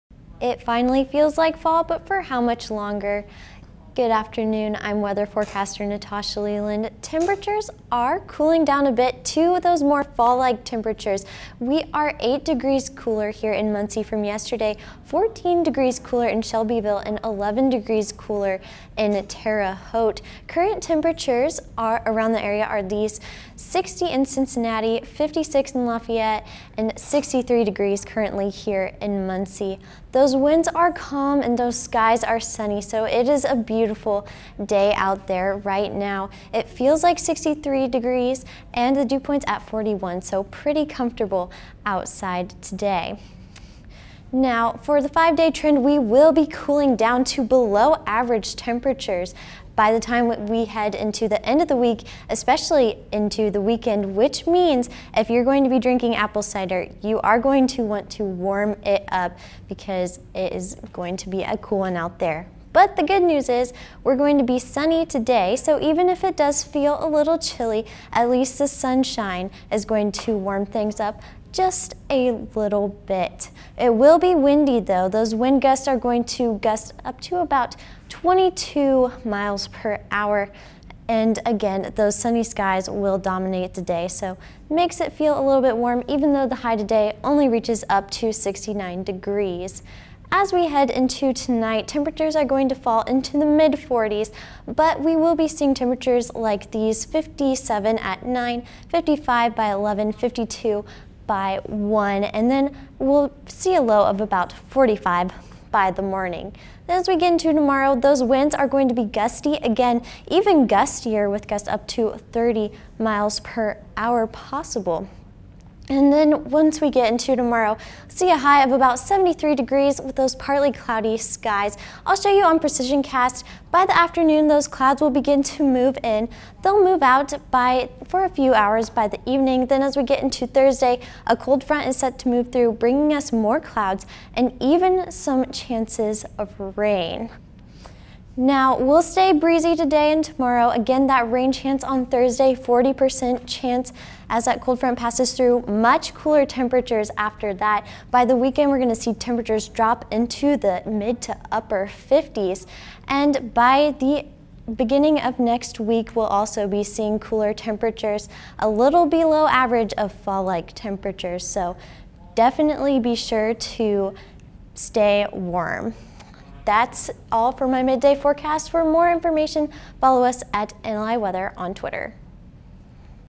NewsLink Indiana Weather October 13, 2020 - Natasha Leland
Weather Forecaster Natasha Leland has updates on cooler temps coming.